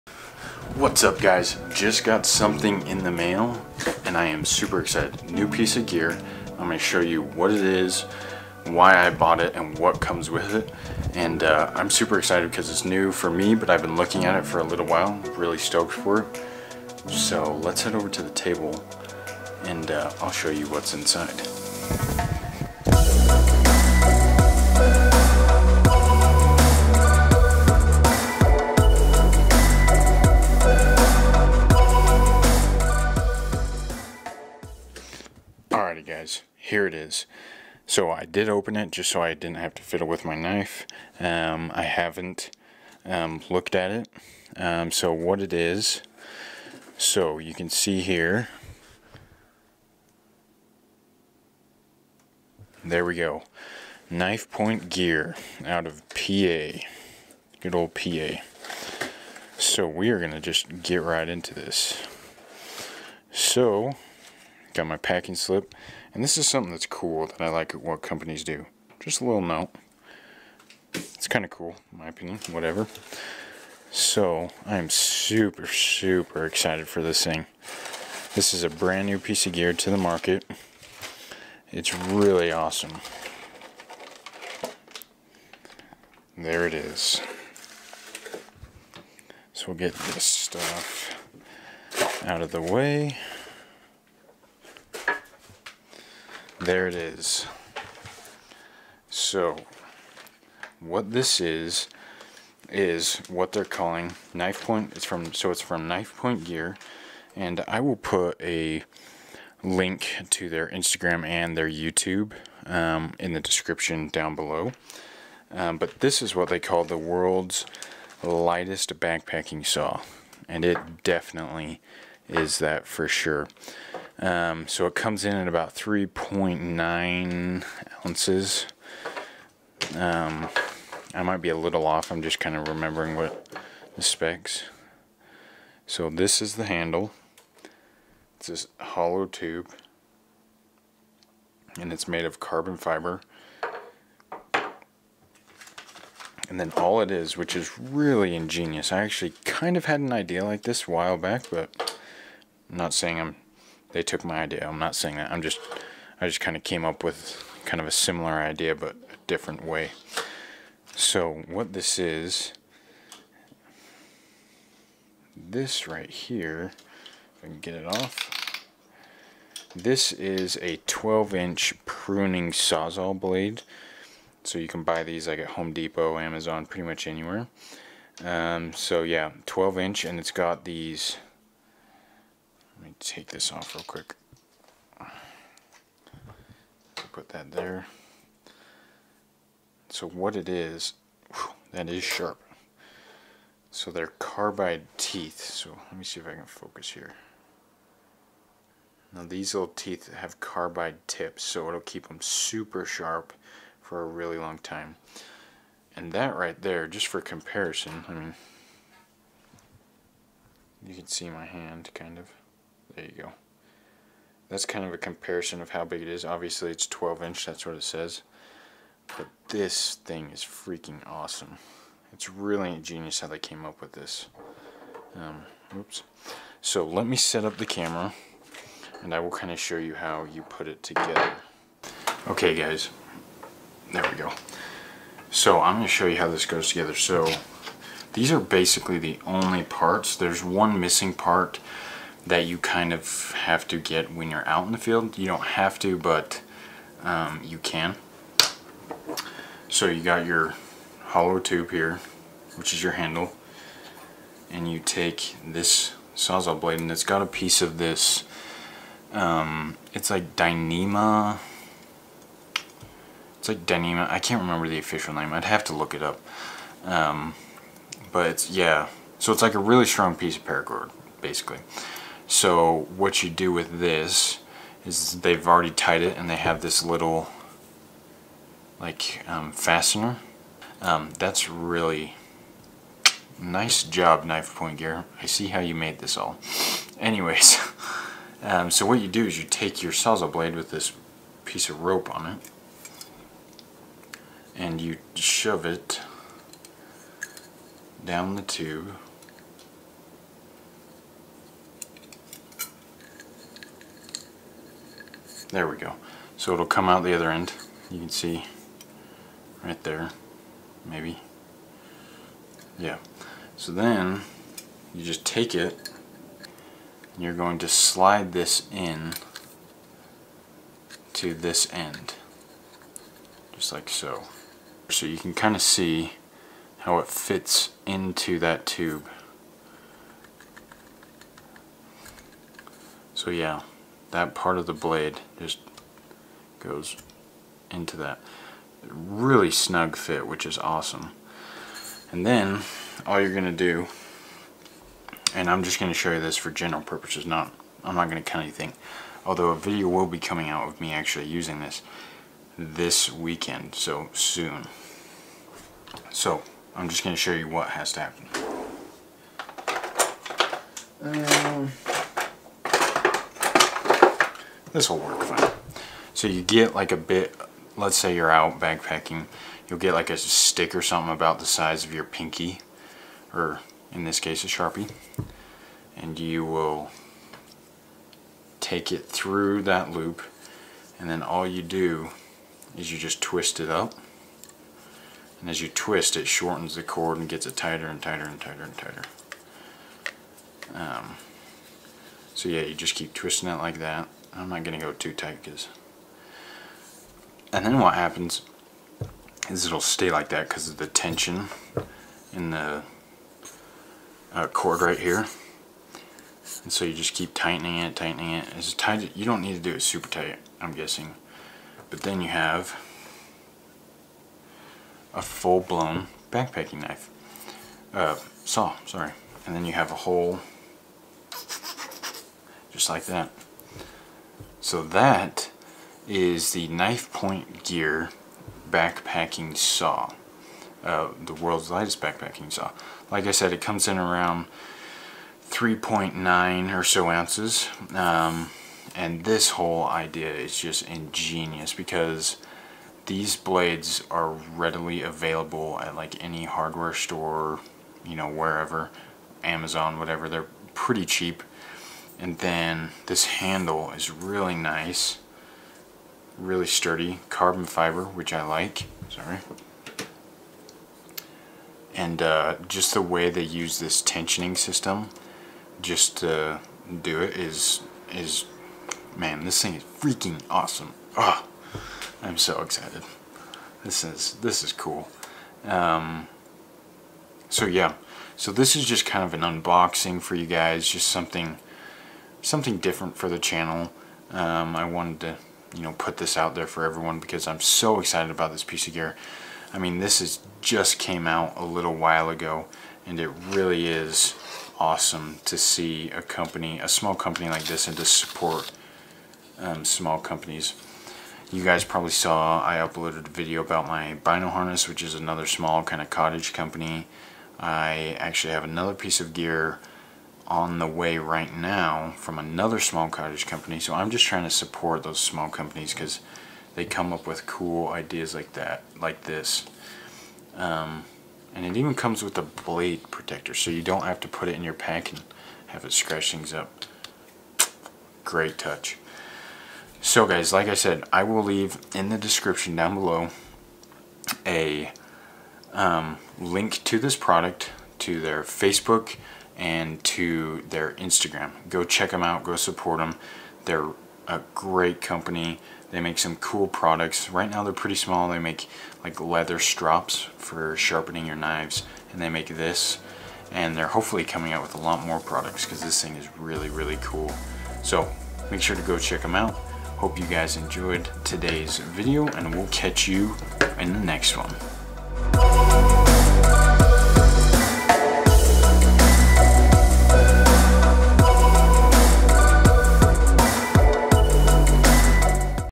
Unboxing The Worlds Lightest Backpacking Saw
The WORLDS LIGHTEST BACKPACKING SAW is only 3.95 oz. I go into what is in the box and how to put together the saw when you purchase the saw for knifepointgear.com. This is the coolest and most unique design i have ever seen make sure to go check out the website to pick up you own.